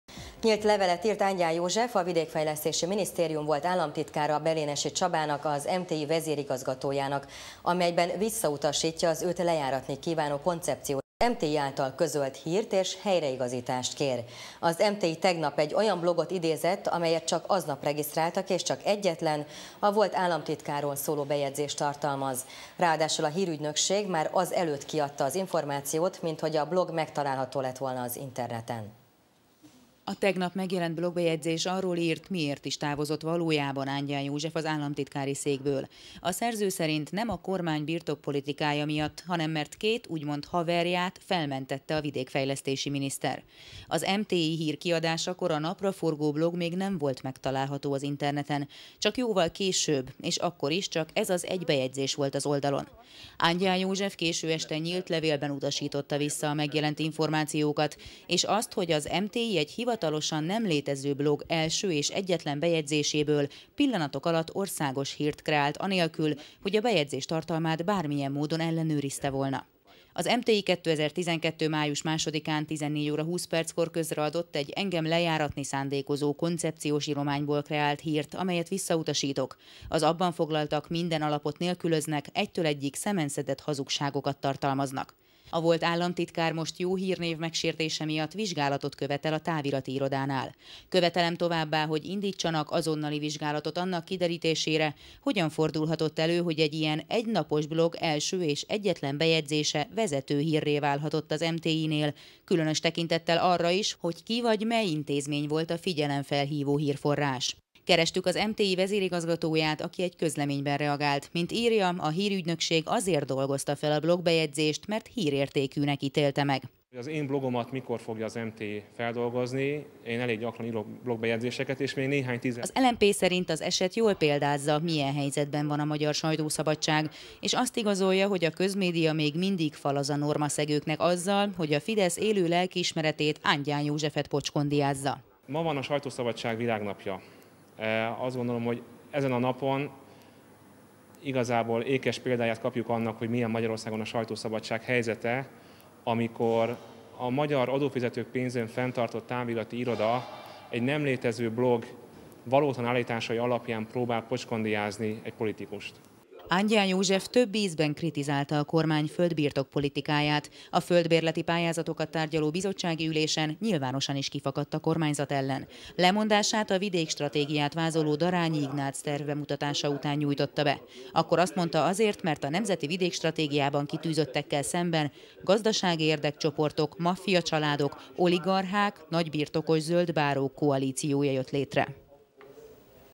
Ángyán: szemenszedett hazugság az egynapos blog állítása
Szemenszedett hazugságnak nevezte a Vidékfejlesztési Minisztérium volt államtitkára azt a szerdai blogbejegyzést, amely azzal vádolta meg, hogy személyes érdekek miatt kifogásolta a kormány birtokpolitikáját.